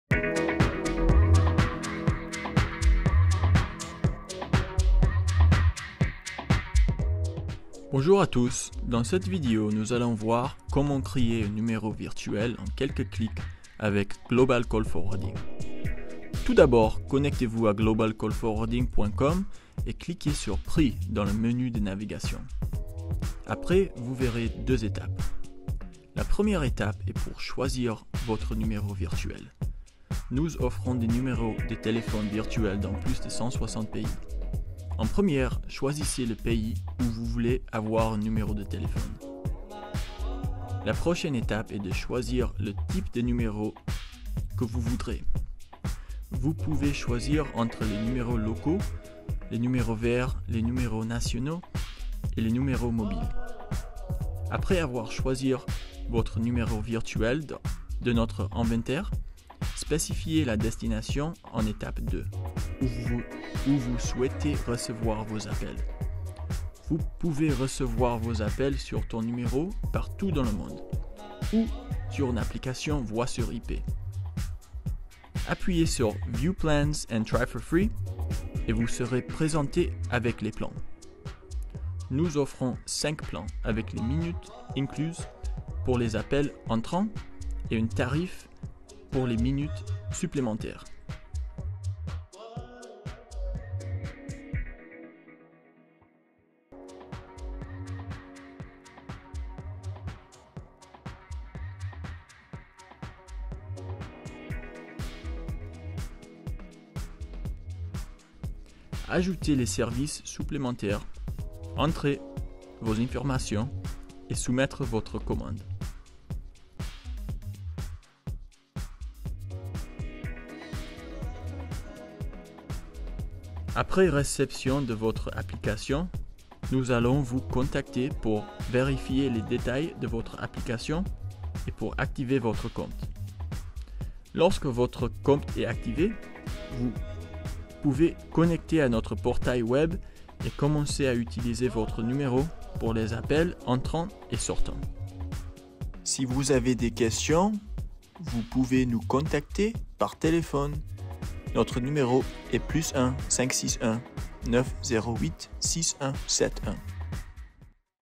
Decouvrez comment créer un numéro virtuel en quelques clics avec Global Call Forwarding
Global Call Forwarding offre des numéros virtuels dans plus de 160 pays.

Un numéro de téléphone virtuel est un numéro qui n’est pas attaché à une ligne téléphonique ou à une carte SIM. Les numéros virtuels sont hébergés dans le cloud, et ils peuvent être utilisés de n’importe ou.

Vous pouvez créer un numéro de téléphone virtuel sur notre site Web en quelques clics. Choisissez simplement un numéro et un plan et complétez votre commande.

Lorsque vous créez votre numéro virtuel, vous pouvez passer et recevoir des appels. Vous pouvez aussi configurer les fonctionnalités avancées. Notre système téléphonique virtuel offre ces fonctionnalités et plus:

- SVI: Serveur Vocal Interactif
- Routage des Appels
- Messagerie Vocal
- Présence Locale
- Extensions
- Routage Horaire
- Enregistrement des Appels

Pour créer un numéro virtuel, suivez ces étapes:

1. Connectez-vous a globalcallforwarding.com et cliquez sur prix dans le menu de navigation.

2. Choisissez le pays où vous voulez avoir un numéro virtuel, choisissez le type de numéro, et choisissez le numéro que vous voulez.

3. Spécifiez la destination ou vous souhaitez recevoir vos appels.

4. Choisissez un plan.

5. Ajoutez les services supplémentaires.

6. Entrez votre information et soumettre votre commande.

Après avoir passé votre commande, nous allons vérifier vos informations et créer votre numéro virtuel. Vous recevrez des informations par e-mail et vous pourrez commencer à utiliser votre nouveau numéro de téléphone virtuel rapidement.